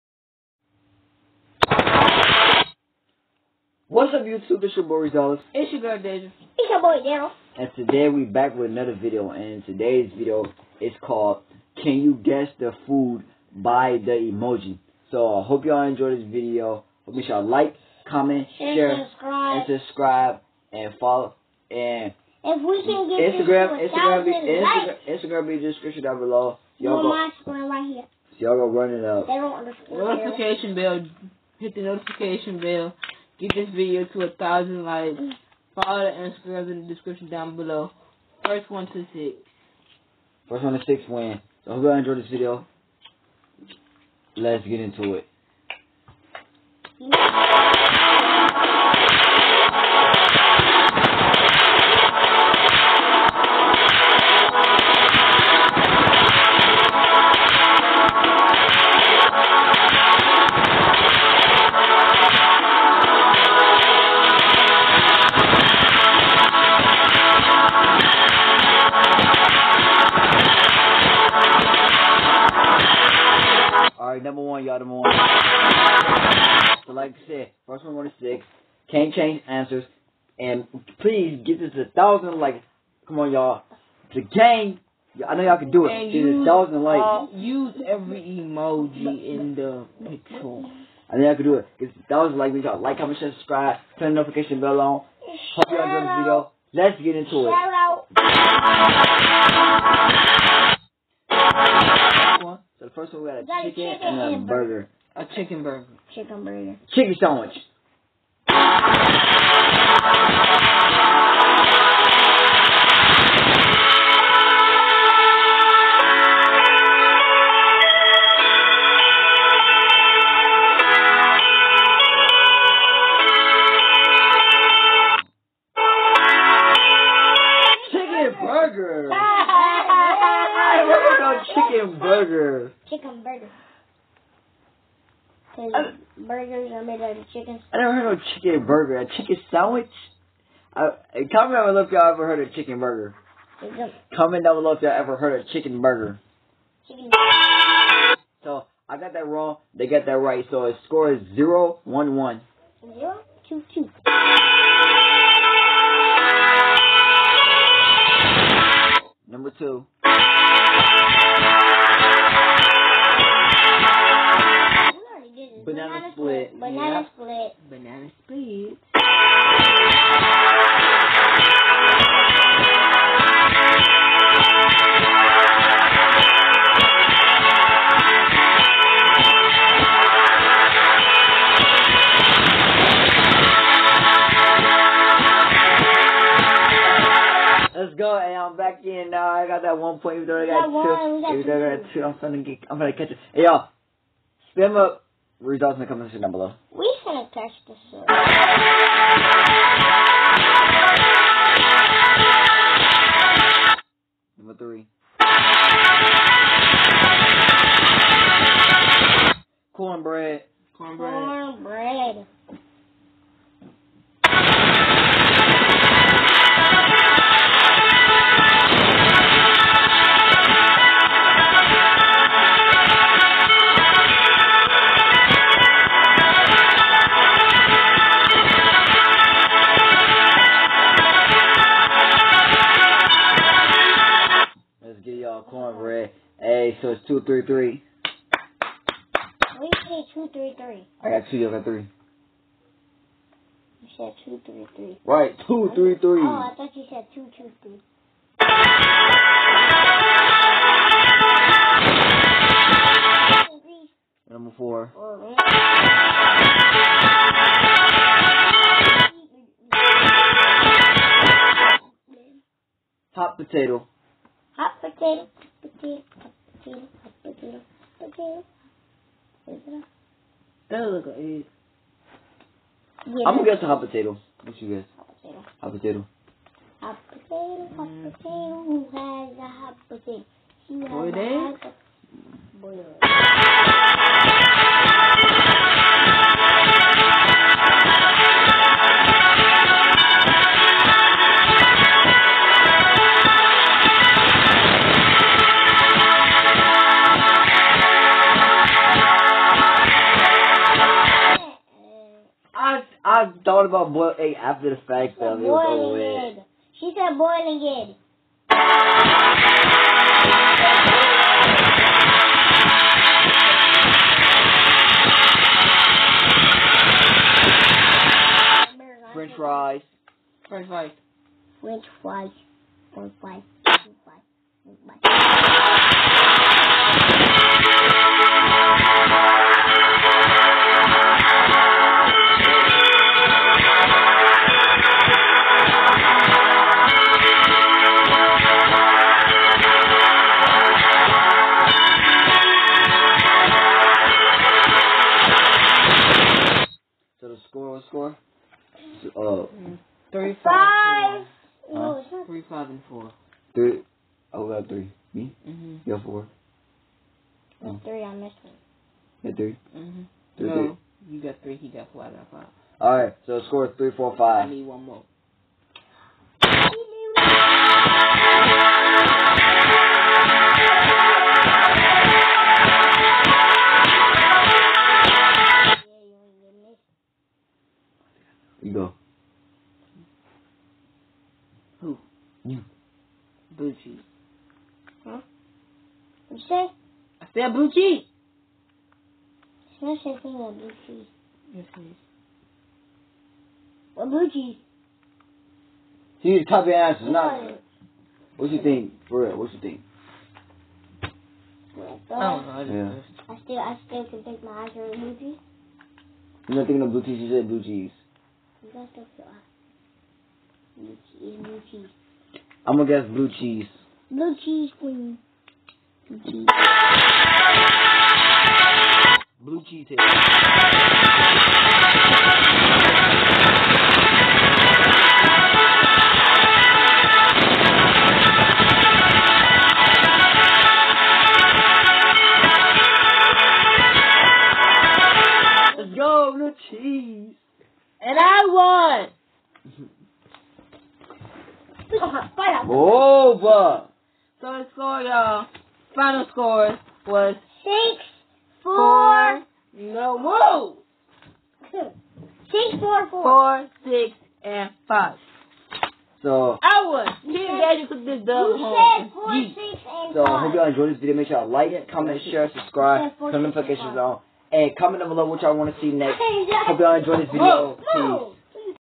Reese Dollars/Daja/Darryl-can you guess the food from the emoji challenge
Reese Dollars Instagram is @reesedollars98
Daja Instagram is @daja_is_taken_period
Daja Instagram is @hair_is_daja